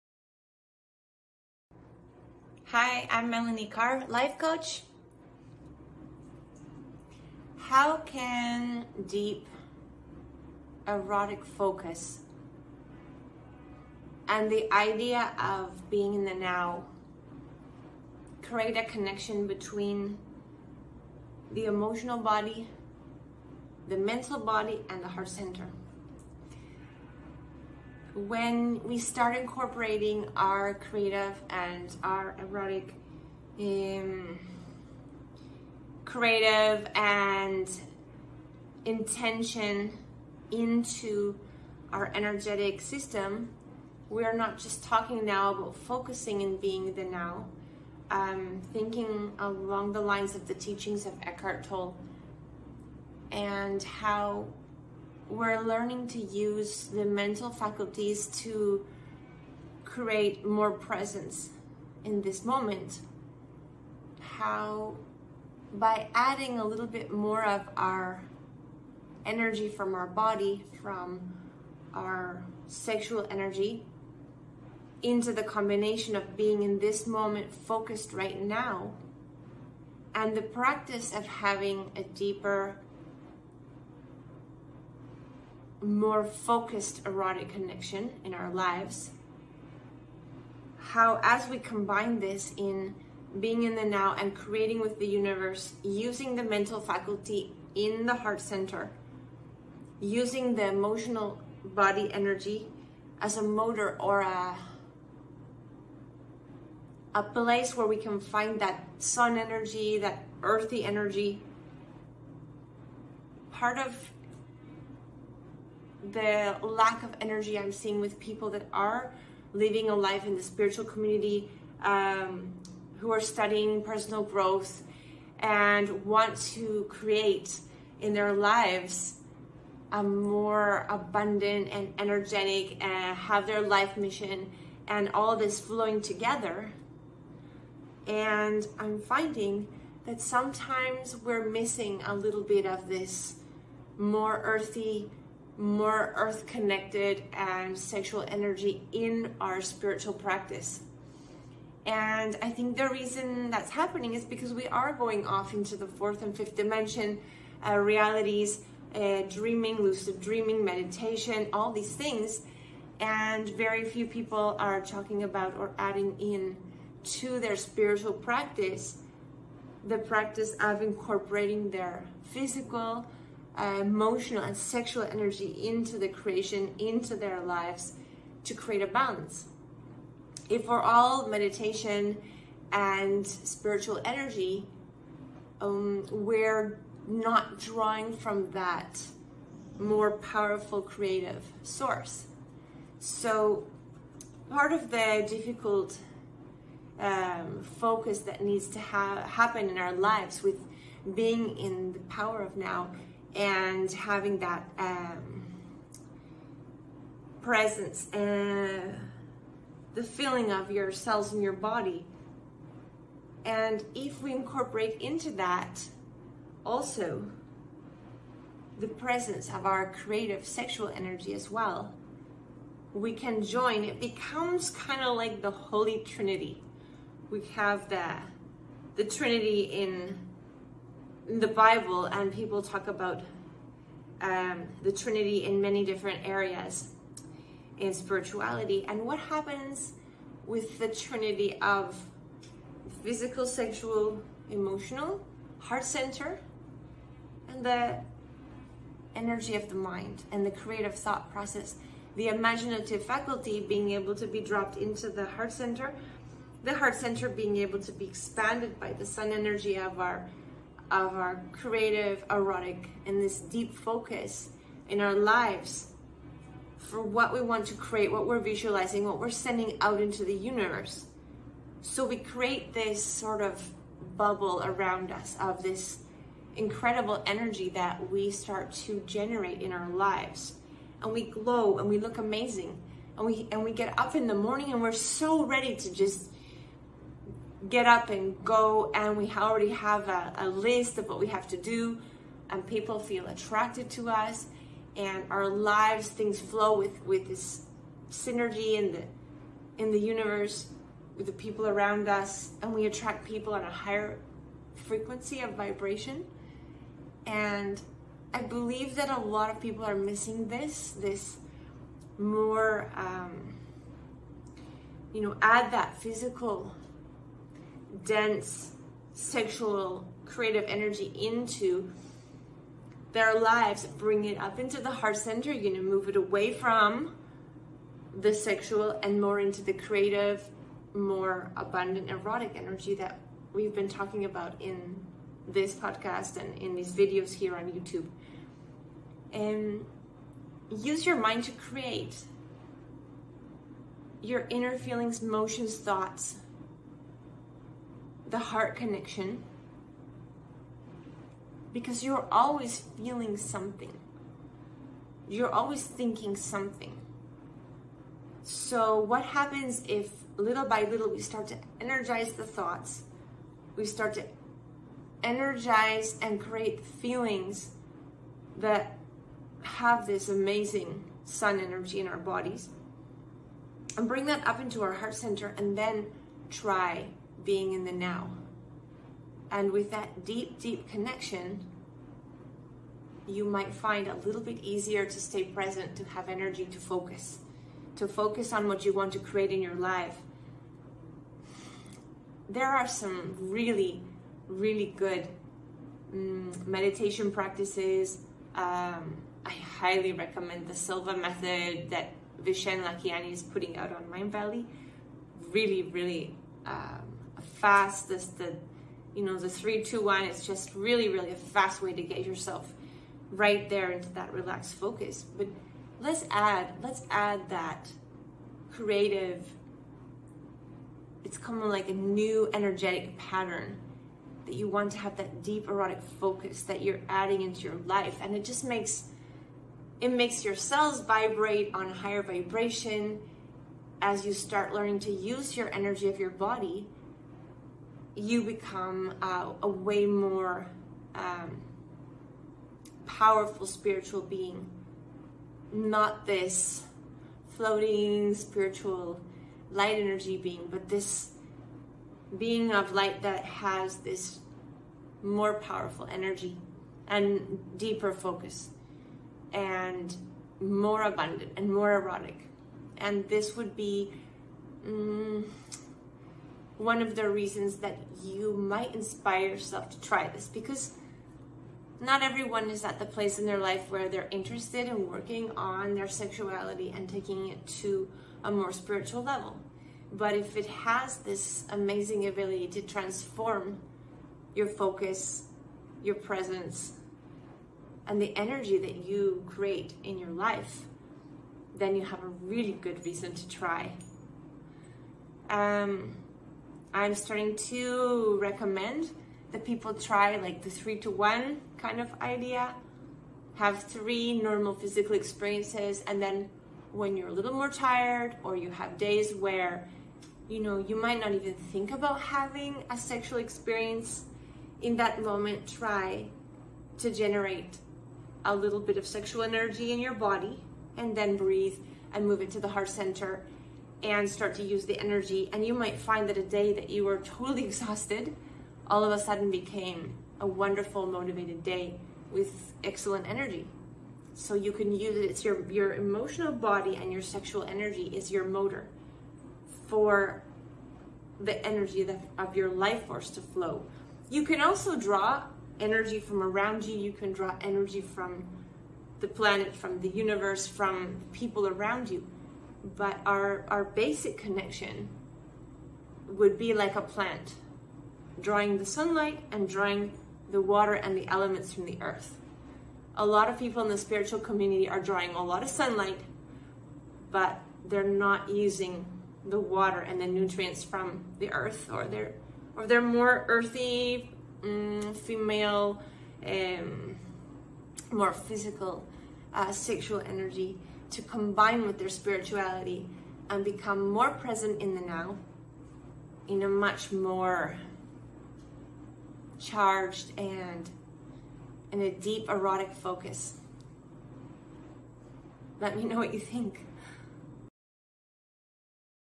DEEP EROTIC/The Power of now
Bring more energy and focus into your Mindfullness practice by using your erotic creative sexual energy to energize your meditation and personal growth practices.